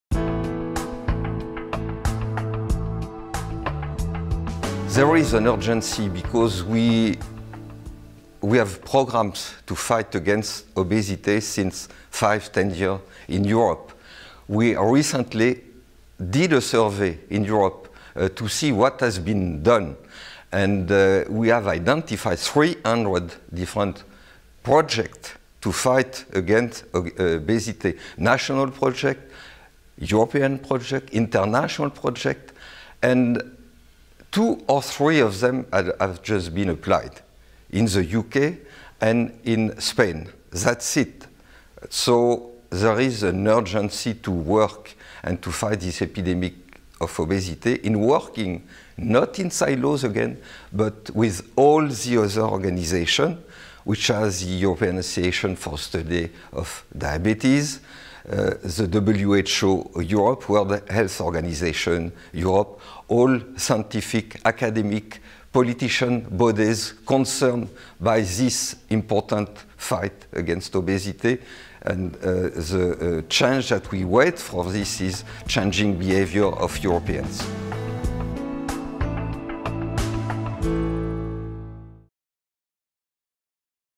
How severe is the obesity problem in Europe?
Video by Jean-Claude Coubard, Executive Director, International Chair on Cardiometabolic Risk, Centre de recherche de l'Institut universitaire de cardiologie et de pneumologie de Québec, Université Laval, Québec City, Québec, Canada

Produced by the International Chair on Cardiometabolic Risk